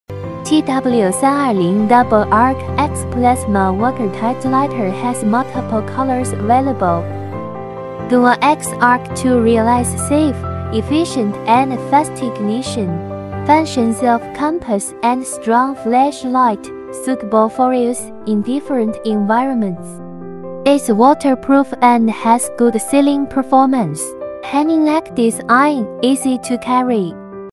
Waterproof Electric Plasma Arc Lighter for Hiking
TW-DHQ-320 is a multi-purpose arc lighter that can be used many times. It can output high-frequency and high-voltage electricity. When it’s working, it produces a light purple arc without crackling,and the temperature is very high,it can easily ignite cigarette, paper, gas stove, Aromatherapy,candle, firework etc.
The product is equipped with a USB charging cable, which can charge the lighter according to your needs.